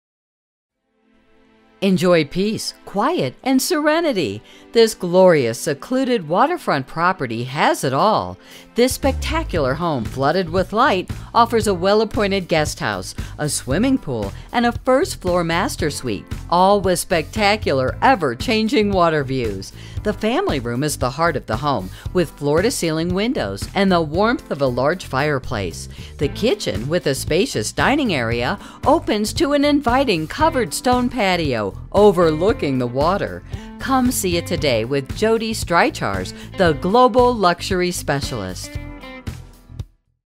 66-7 Ely Ferry Road/Tinker Lane Lyme, CT | ColdwellBankerHomes.com
66-7 Ely Ferry Road/Tinker Lane , Lyme, CT

PEACE, QUIET AND SERENITY. This glorious secluded waterfront property has it all! Featuring a spectacular home that is flooded with light from walls of windows, a well-appointed guest house, a swimming pool and a first-floor master suite, all with spectacular ever-changing water views. Perfectly sit